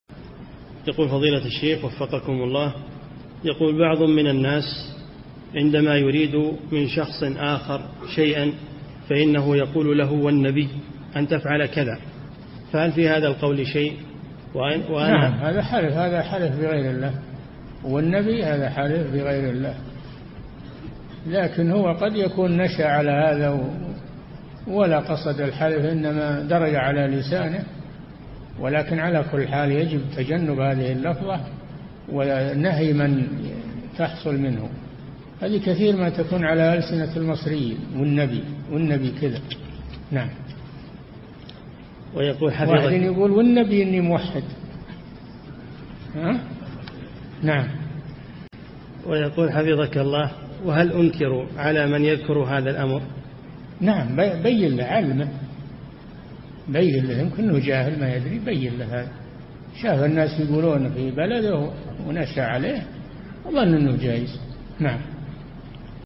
[53 -939] ما حكم الحلف بقول "والنبي أن تفعل كذا"؟
-[53 -939] السؤال: بعض من الناس عندما يريد من شخص آخر شيئا، فإنه يقول له: والنبي أن تفعل كذا، فهل في هذا القول شيء؟
الجواب: نعم هذا حلف بغير الله، والنبي هذا حلف بغير الله، لكن هو قد يكون نشأ على هذا ولا قصد الحلف إنما درج على لسانه ولكن على حال يجب تجنب هذه اللفظة ونهي من تحصل منه هذا كثيرًا ما تكون على ألسنة المصريين والنبي والنبي كذا واحد يقول والنبي أني موحد! السائل: هو يقول حفظك الله، وهل أنكر على من يذكر هذا الأمر؟، الشيخ: نعم بيِّن له علِّمه بيِّن له، يمكن أنه جاهل ما يدري بيِّن له هذا، شاف الناس يقولونه في بلد ونشأ عليه وظن أنه جائز.
للاشتراك في قناة الشيخ صالح الفوزان

للاستفسار والإقتراحات والدعم
رئيس الفريق
لمشروع كبار العلماء